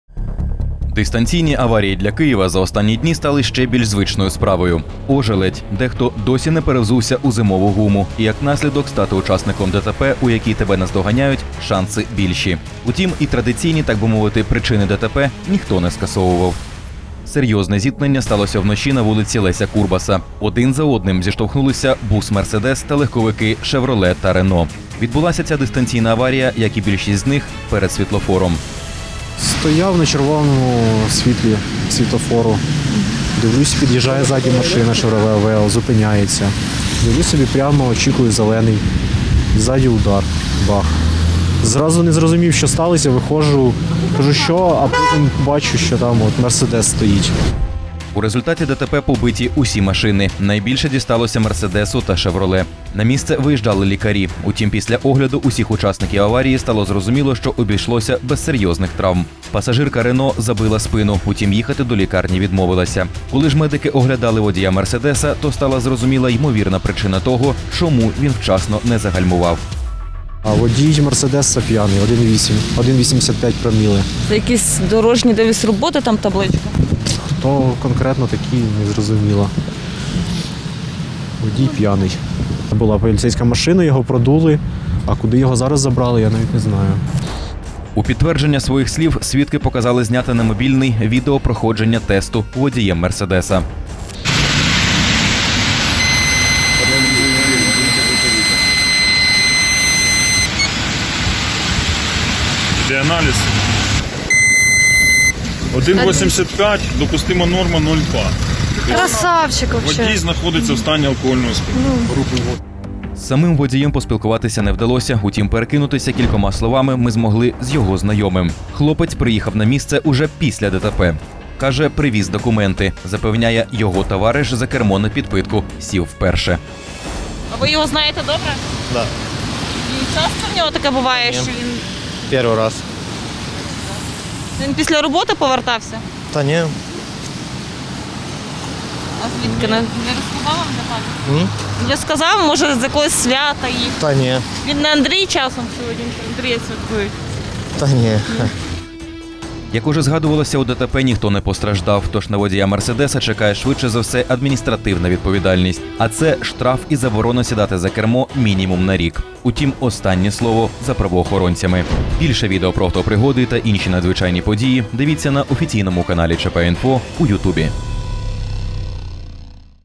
В Києві п’яний водій буса влаштував масову ДТП
Серйозне зіткнення сталося вночі на вулиці Леся Курбаса. Один за одним зіштовхнулися бус Мерседес Шевроле та Рено. Відбулася ця дистанційна аварія, як і більшість з них перед світлофором.